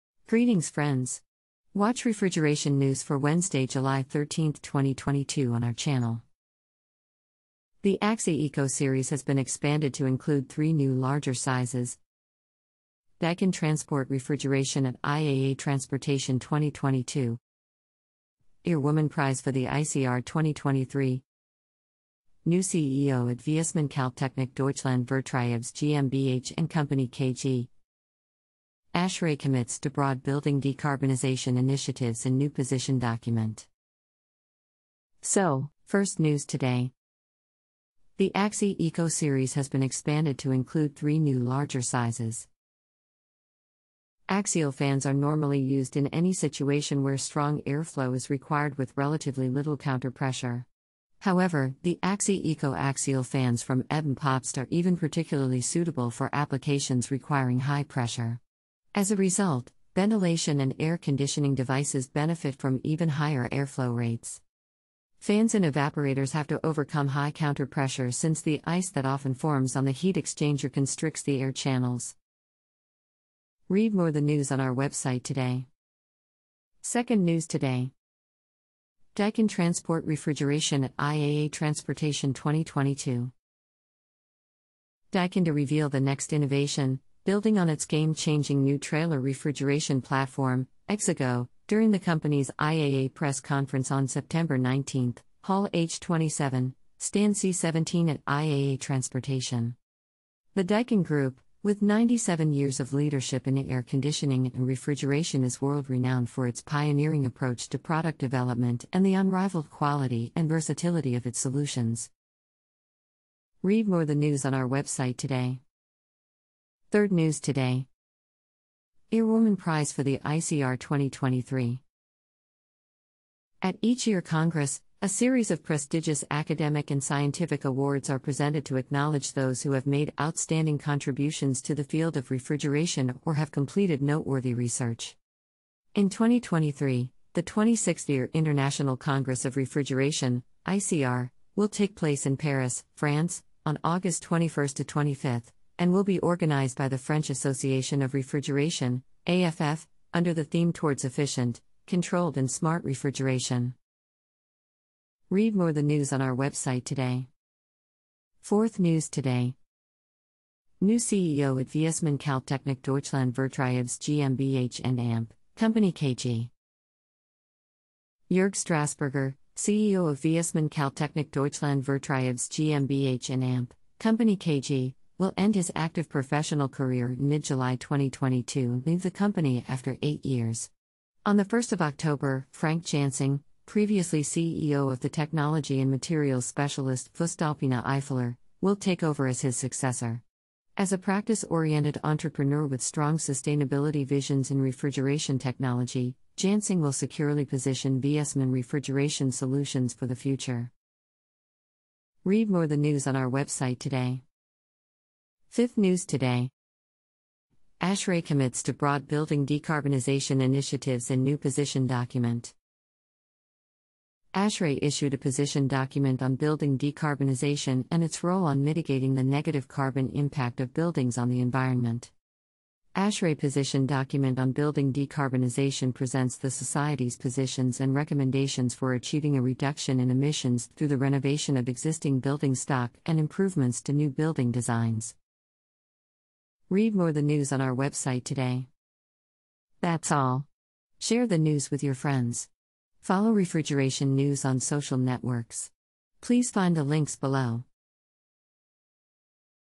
The AxiEco series has been expanded to include three new larger sizes.
Watch refrigeration news for Wednesday, July 13, 2022.

Today news:
00:35 The AxiEco series has been expanded to include three new larger sizes.
01:16 Daikin Transport Refrigeration at IAA Transportation 2022.
01:56 IIR Woman prize for the ICR 2023.
02:38 New CEO at Viessmann Kältetechnik Deutschland Vertriebs GmbH & Co. KG.
03:25 ASHRAE Commits to Broad Building Decarbonization Initiatives in New Position Document.

Follow news, buy and sell refrigeration equipment on the web portal Refrigerating industry